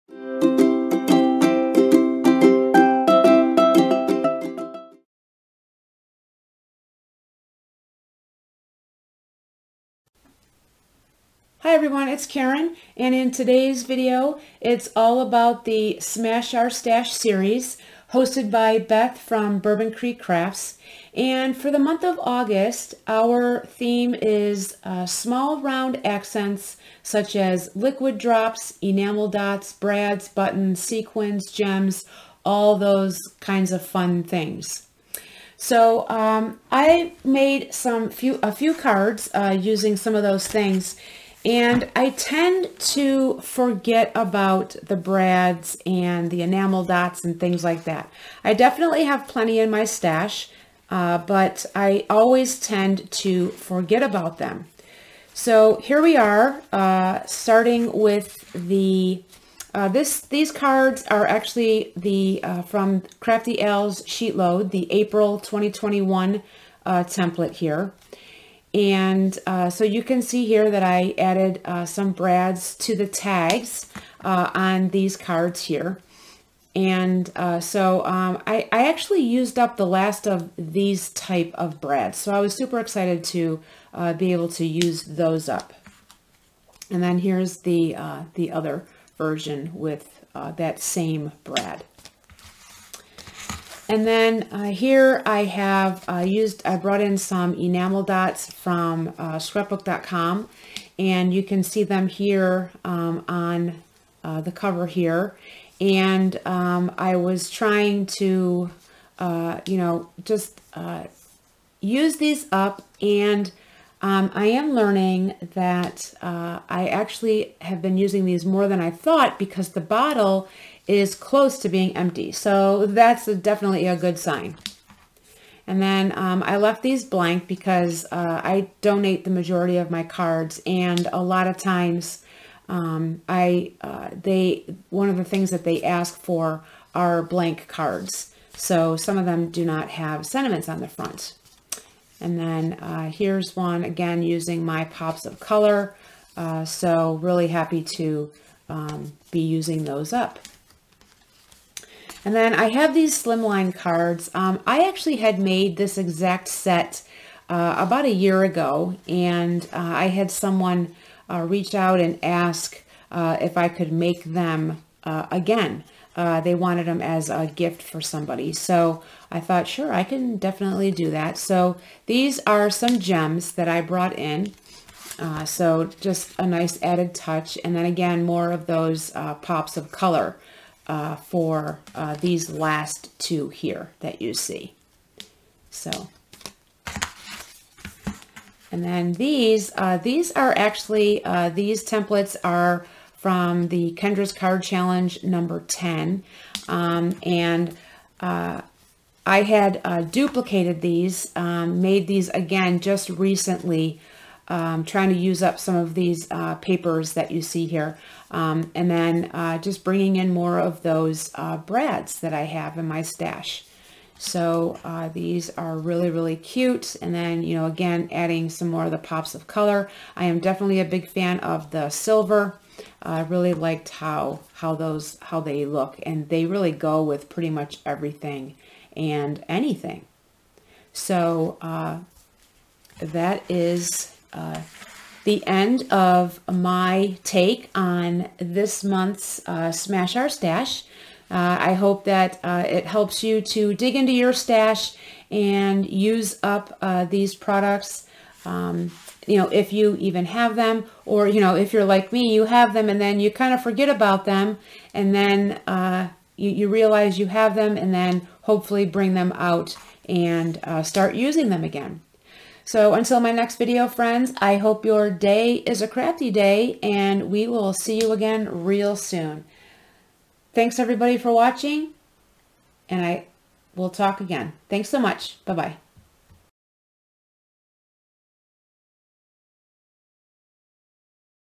Smash Our Stash August 2023 - Using small round accents
Hi everyone! See the cards I made using some eyelets, enamel dots, gems, pops of color and brads. As we are working on smashing our stash in 2023, we hope we an inspire you to wok on using up your stash this year as well.

I hope your day is a crafty day!

Hugs,
Karen